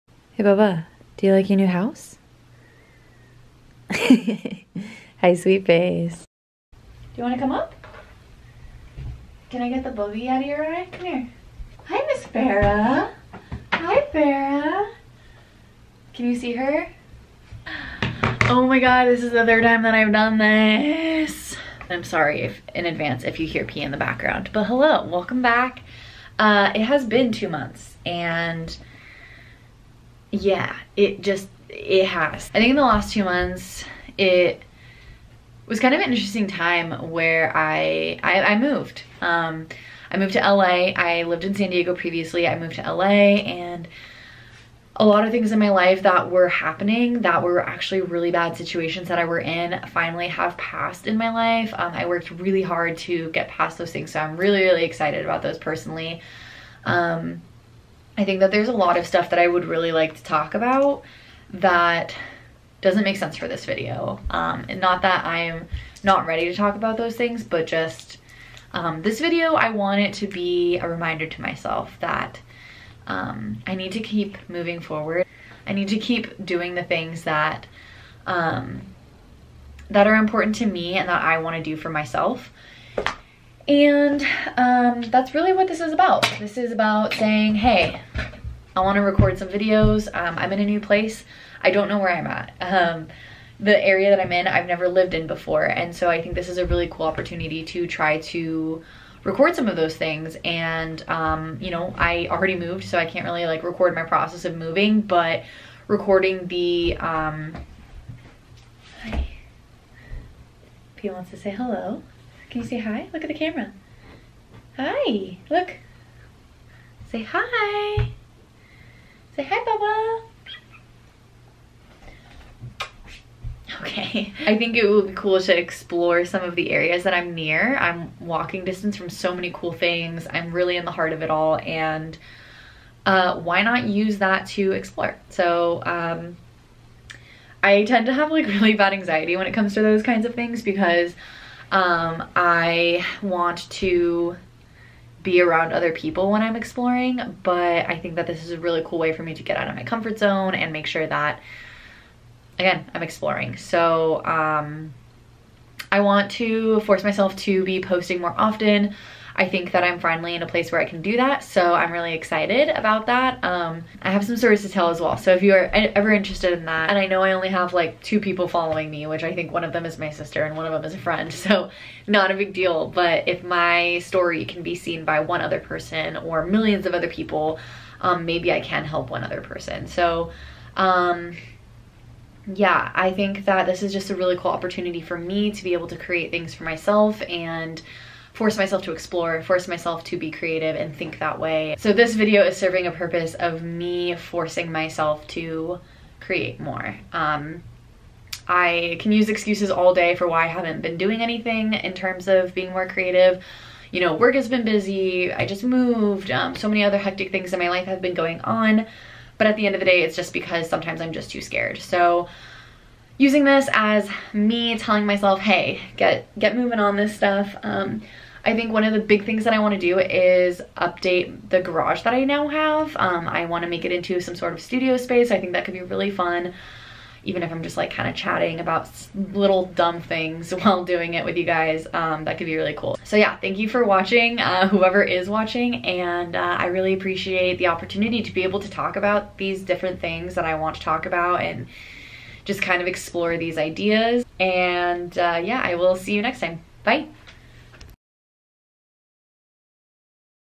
I Moved! | Jess Capó
Hello hello!

I moved, and took 2 months away from being creative.